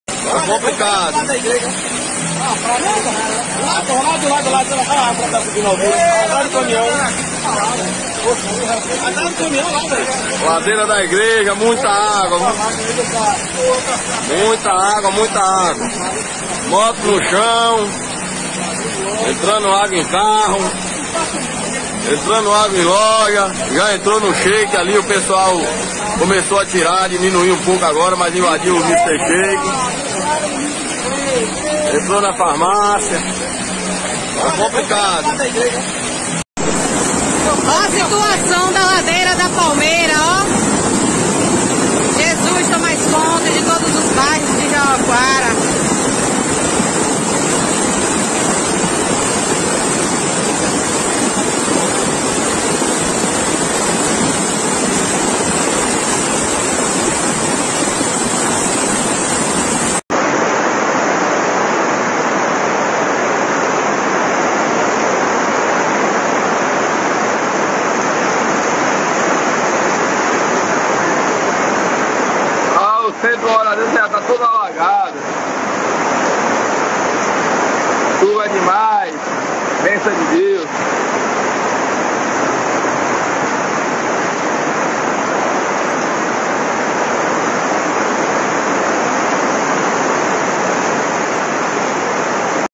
Chuva forte em Jaguaquara causa alagamentos e deixa estragos| hoje
Chuva em Jaguaquara Bahia. Causa alagamentos água invade ruas e deixa estragos na cidade nessa sexta-feira (03/12/2021). O Vídeo mostra temporal em Jaguaquara  (BA), casas e postos comerciais ficaram inundado.